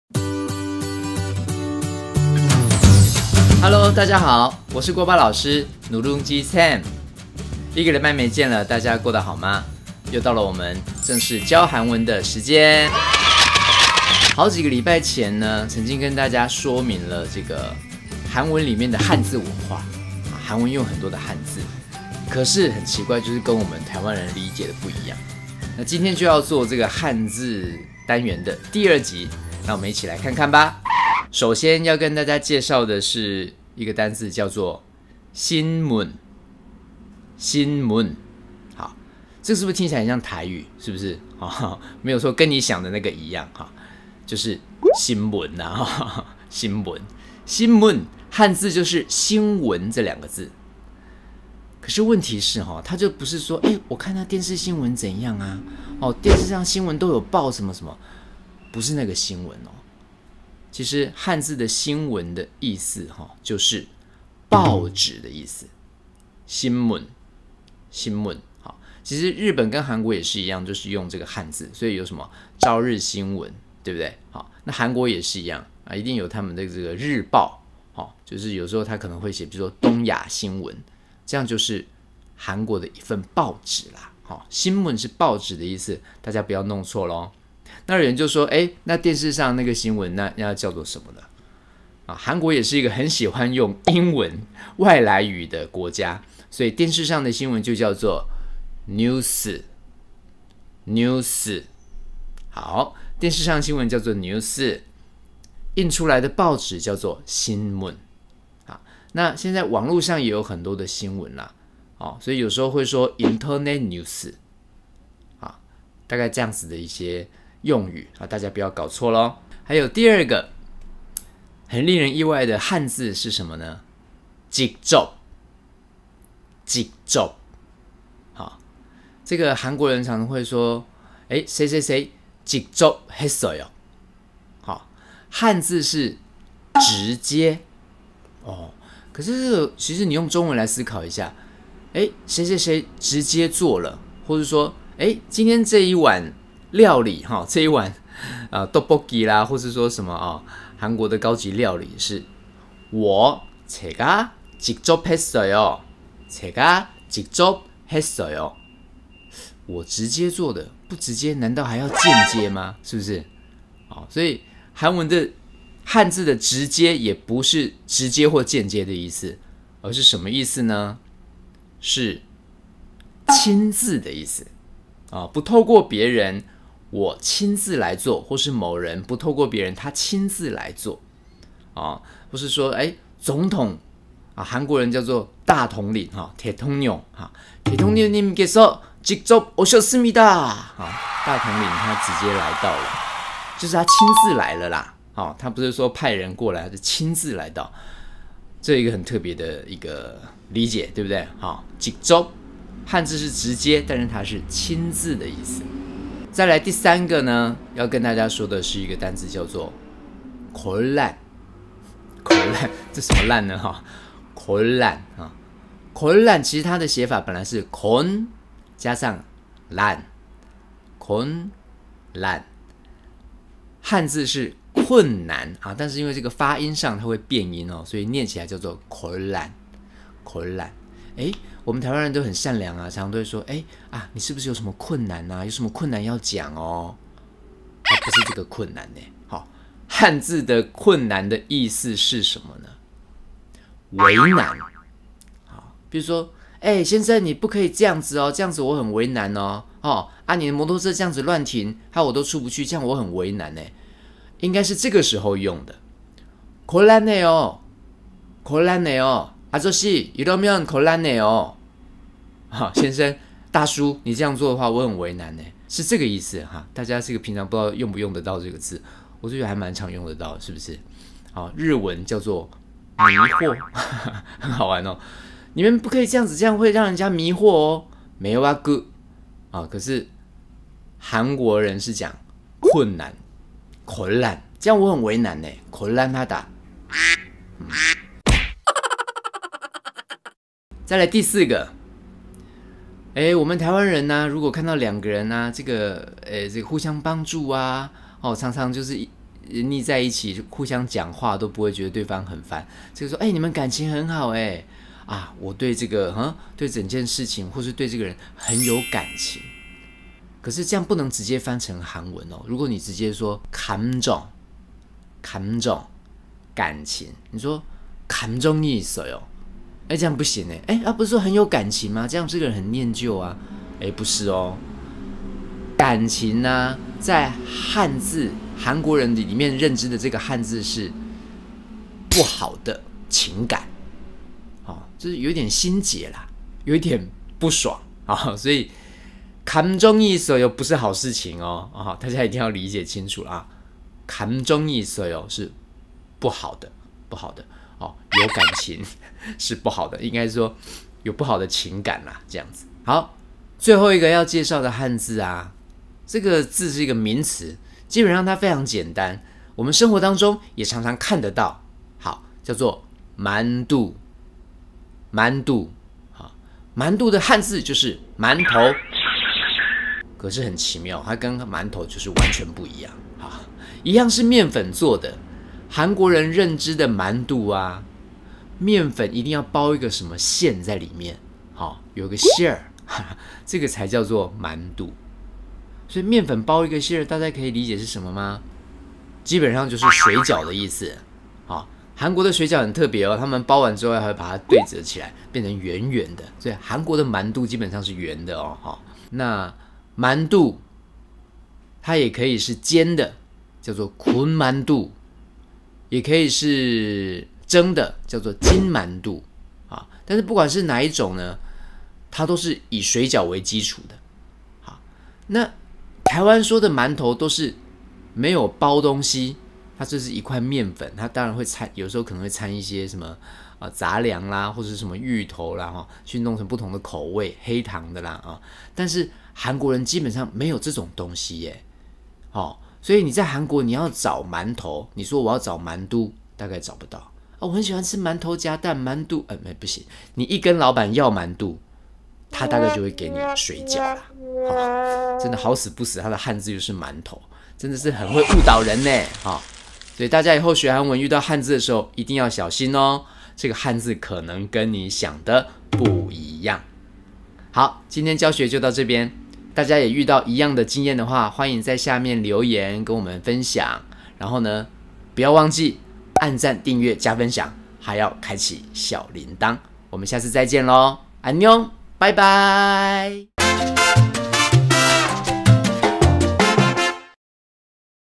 韓文教學｜最令人意外的5大漢字【第2集】來囉！「饅頭直接上新聞，感情困難?!!」到底是什麼意思？？？
這幾週收集了很多個「令人意外」的漢字~~寫在我的小本本裡面，
最後千挑萬選才又選出了「5個」學到懷疑人生的「魔王漢字」！
快來看看你有沒有正確理解它們的意思吧！Go~~~